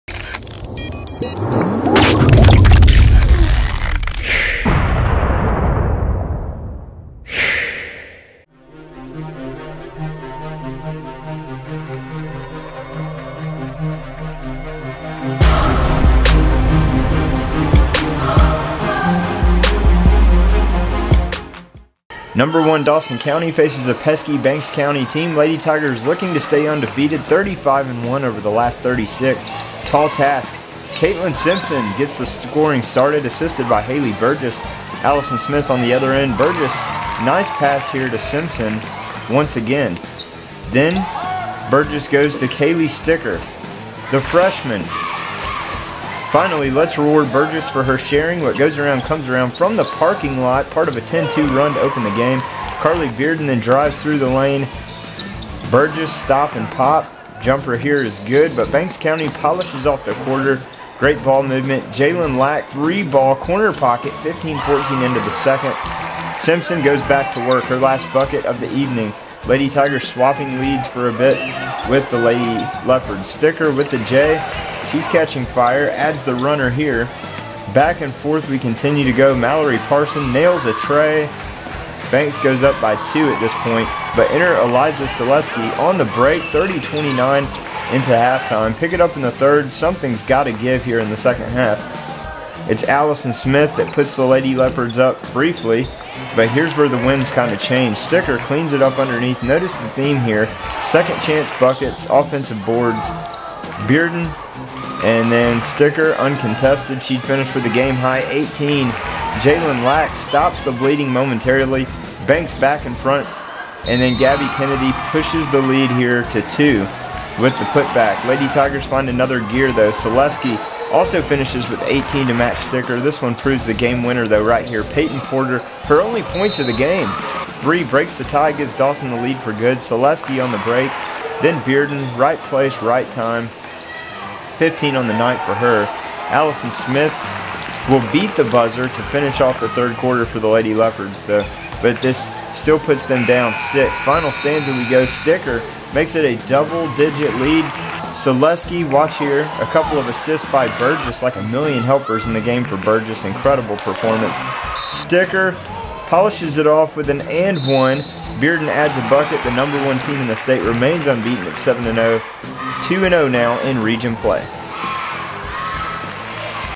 Banks Co Lady Leopards vs #1 Dawson Co Lady Tigers
Banks Co Lady Leopards (5-2; 1-1 7-AAA) #1 Dawson Co Lady Tigers (6-0; 1-0 7-AAA) in Region 7-AAA action from Homer, GA. December 15, 2015. Freshman Kaylee Sticker and senior Eliza Sillesky led the #1 Lady Tigers with 18 pts each, while senior Karlie Bearden added 15. Haley Burgess had 9 pts and countless assists in an incredible performance. Allison Smith led the Lady Leopards with 15 pts, while Taylor Parson added 12. Dawson’s win kept it undefeated at 7-0 and ran its record to 35-1 over the last 36 games.